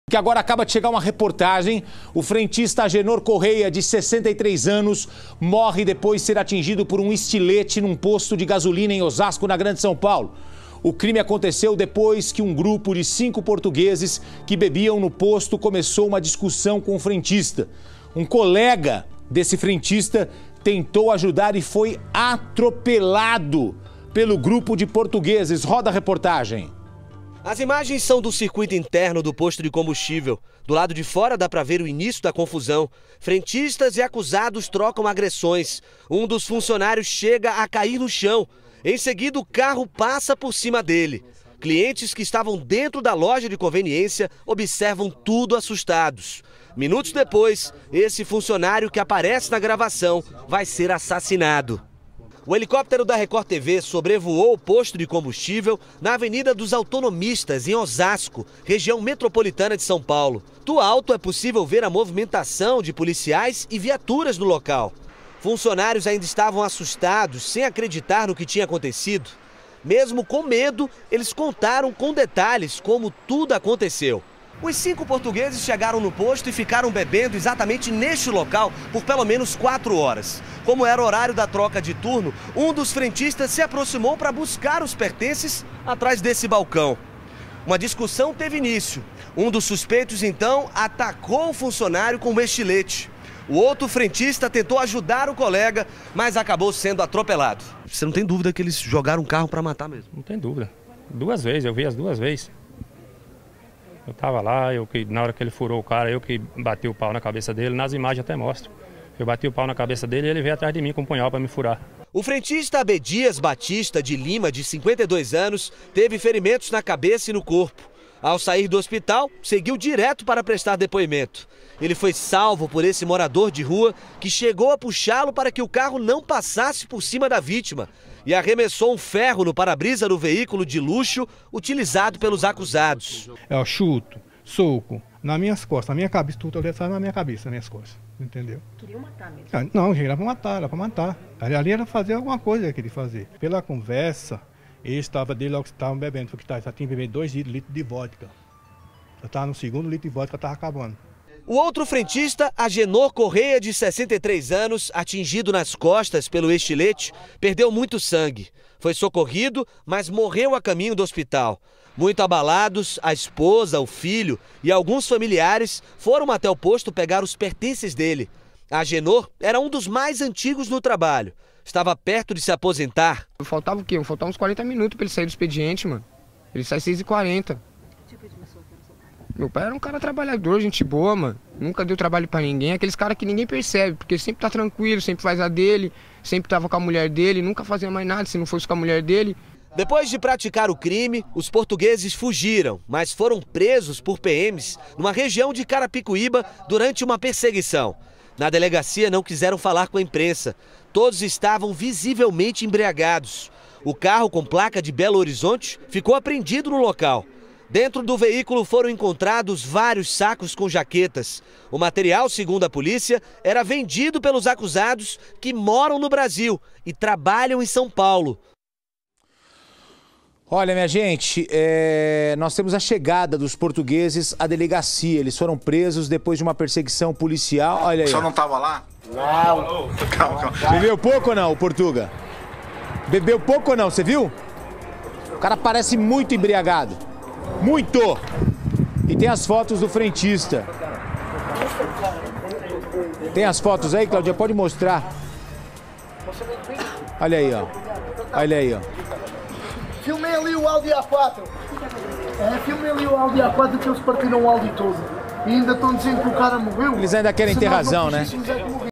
Frentista morre após ser atingido por estilete durante discussão
Cinco portugueses já bebiam havia mais de quatro horas em um posto de combustíveis de Osasco (SP) quando iniciaram uma discussão com os frentistas. Um trabalhador foi atingido por um estilete e morreu a caminho do hospital. Outro ainda foi atropelado claramente de propósito. Os portugueses foram presos em Carapicuíba, visivelmente embriagados. Eles moram no Brasil e trabalham em São Paulo.